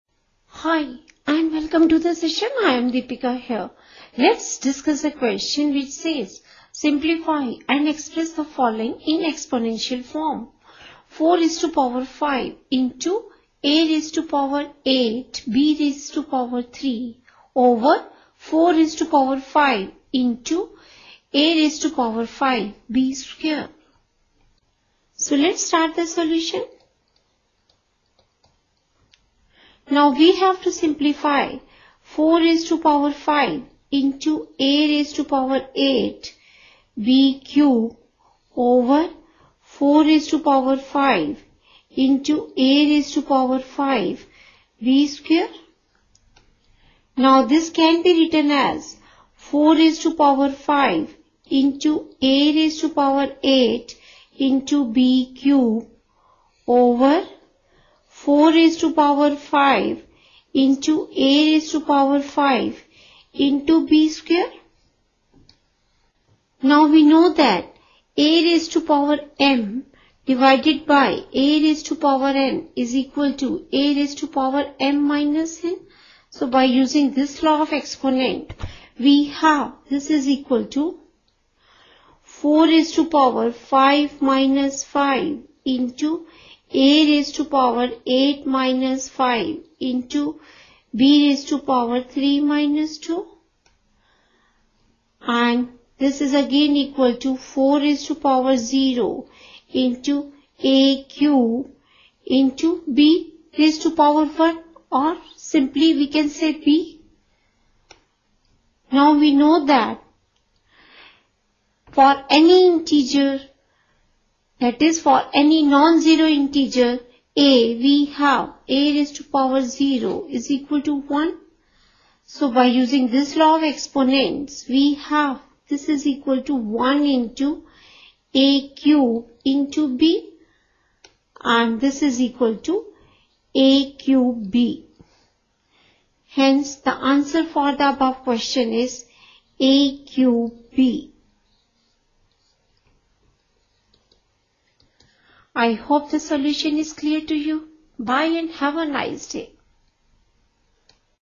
Example: Simplify Using Laws of Exponents
In this example, we show how to simplify a rational  expression involving numbers and variables  by using laws of exponents.